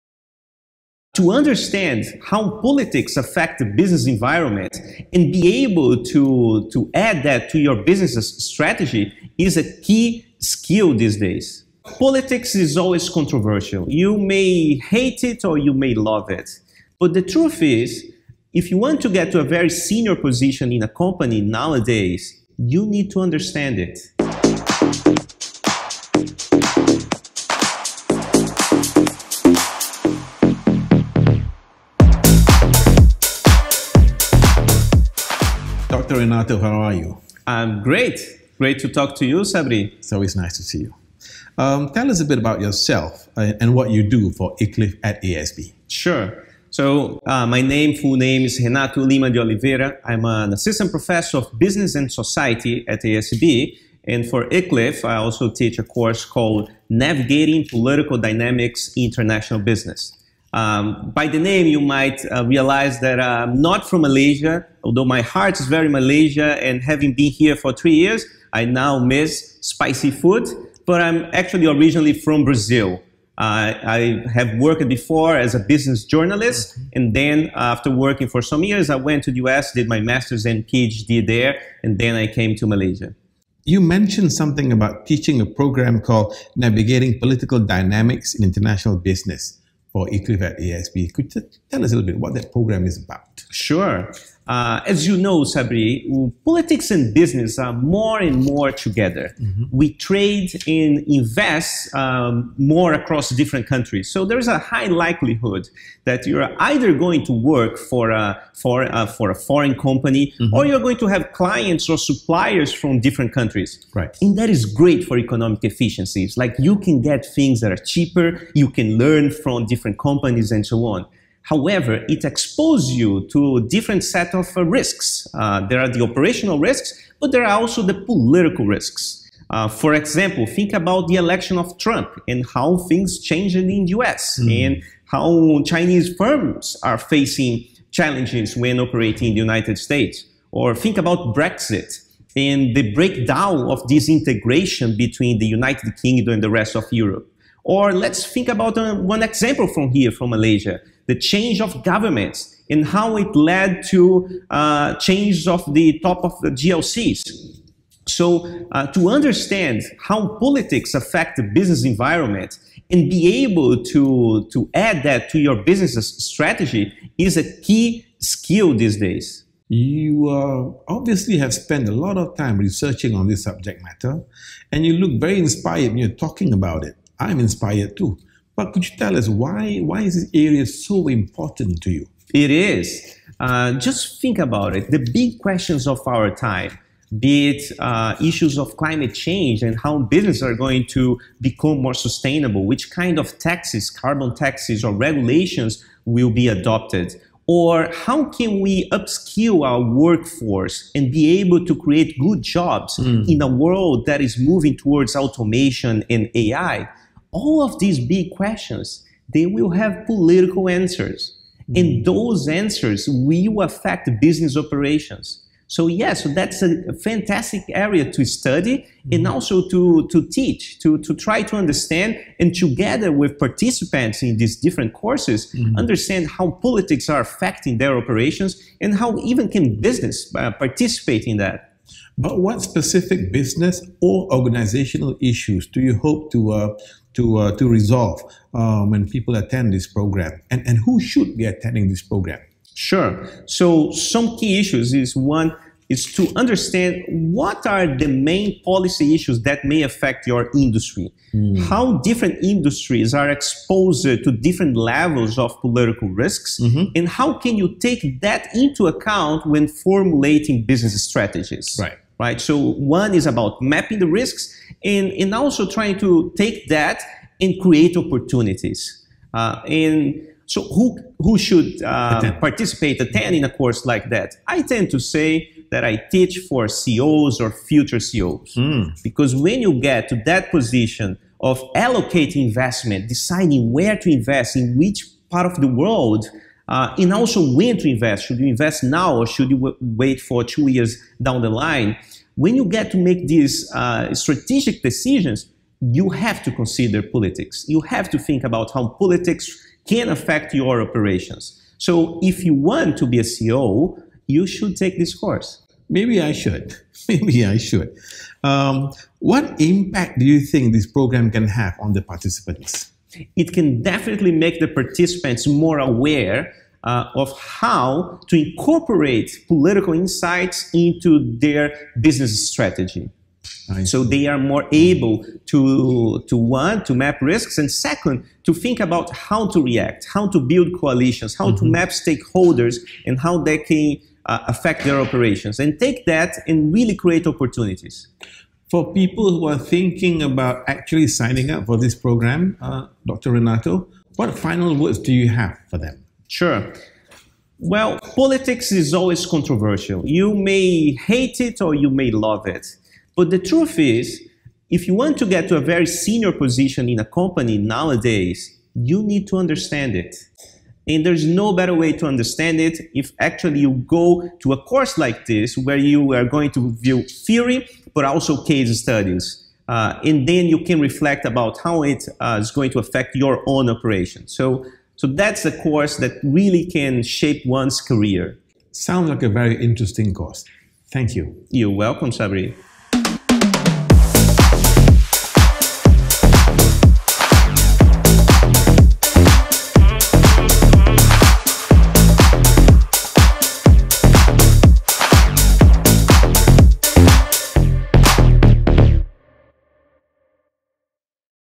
Executive Education | Navigating Political Dynamics in International Business | ASB Iclif
Managing in a global economy requires knowledge of political institutions and incentives that affect business operations such as regulatory changes and expropriations. As a decision maker, you will need to assess and deploy social or non-market strategies.

This professional program, delivered by Professor Renato Lima de Oliveira, Assistant Professor of Business Society at Asia School of Business, seeks to provide participants with frameworks and examples to understand the interaction between politics and the economy, map political risks to investments, and devise strategies to react and reduce risks coming from political factors. In addition to covering frameworks of universal application, we will discuss cases from Southeast Asia, Latin America and Africa.

KEY TAKEAWAYS
1. Understand how to identify political risks to investments that managers operating globally must take into account.
2. Understand how to formulate non-market strategies to gain competitive edge.

You can also explore other Executive Education courses revolving around finance and the global economy as well as other topics relevant to contemporary leaders